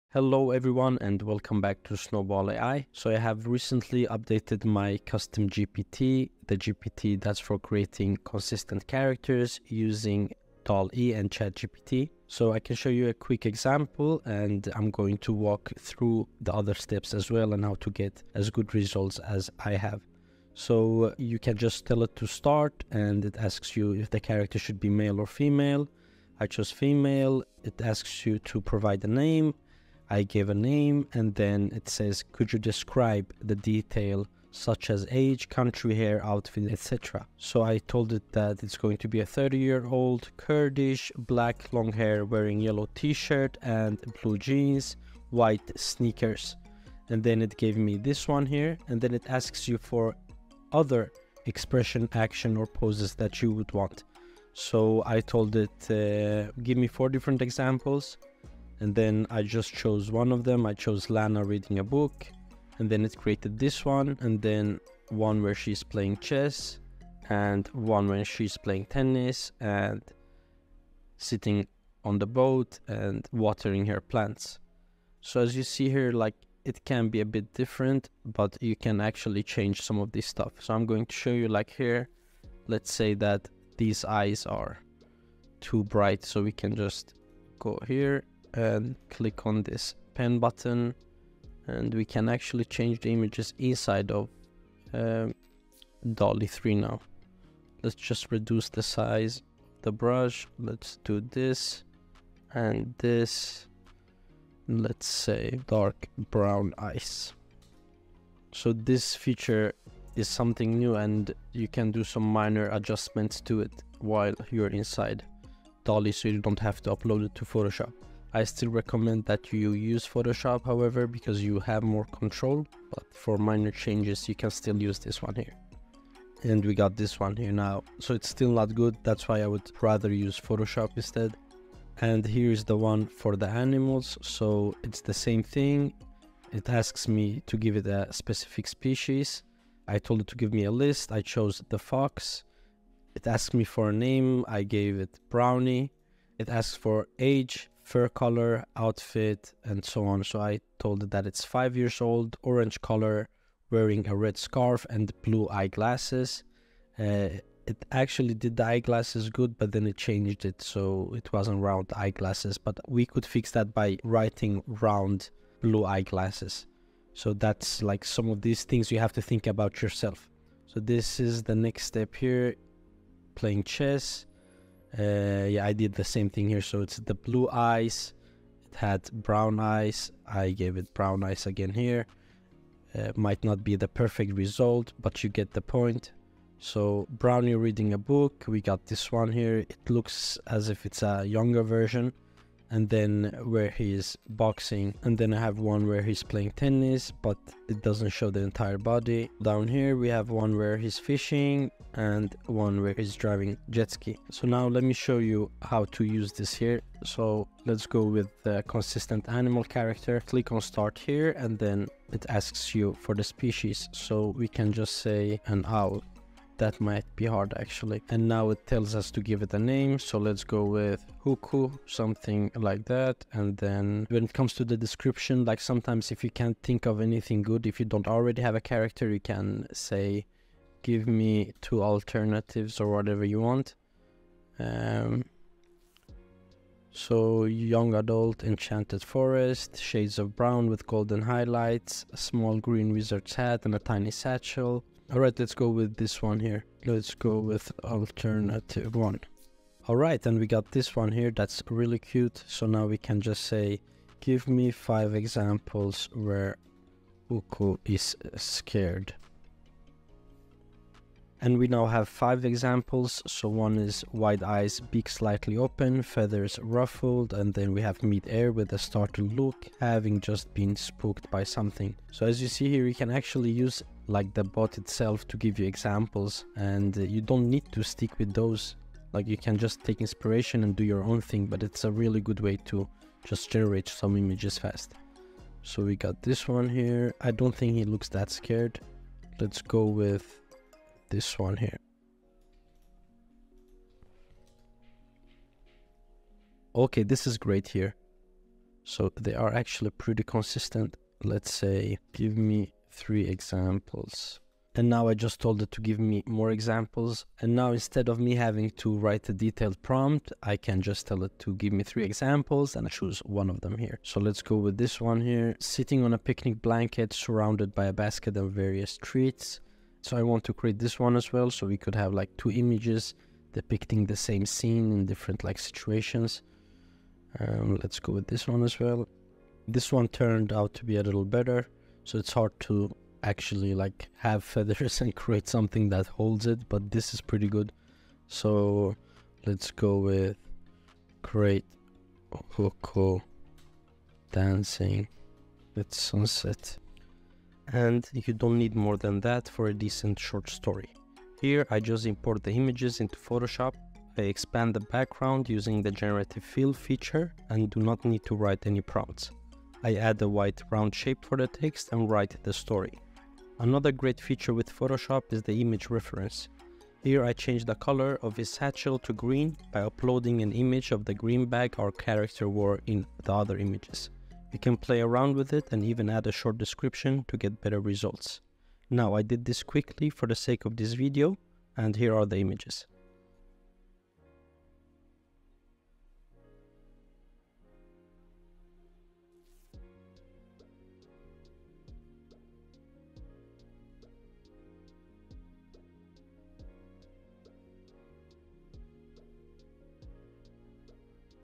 Consistent Character CustomGPT - Improved (Animals + Human)
I've updated my custom GPT for creating consistent characters & animals using DALL-E and ChatGPT.

I've updated my custom GPT for creating consistent characters using DALL-E and ChatGPT. In this video I demonstrate how to generate and customize characters with ease using my CustomGPT.

I start by creating a female character named Lana, detailing her appearance and outfits. I'll show you how to fine-tune features directly within DALL-E, though Photoshop is still recommended for major edits.

Next, I create an animal character, a fox named Brownie, showcasing the flexibility in generating different poses and actions. You'll see how to use the bot to get quick and varied examples, making character creation faster and more efficient.